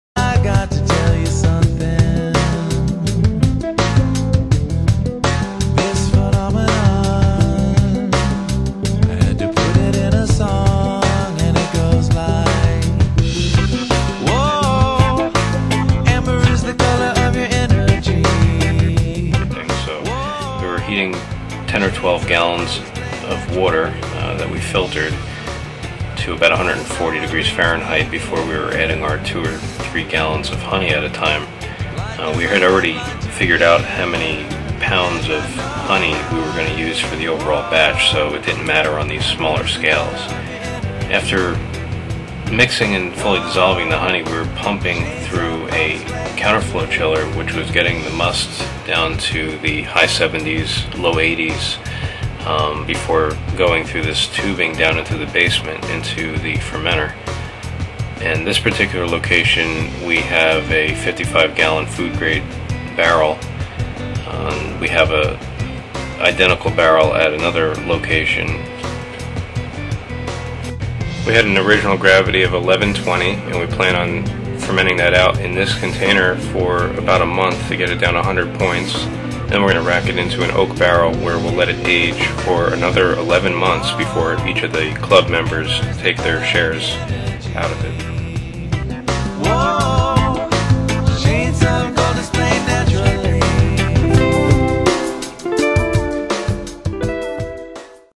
Brewing a lot of Mead
The WHALES homebrew club pooled some money (and honey) together to made 110 gallons of mead. It will be aged in oak barrels for a year and thoroughly enjoyed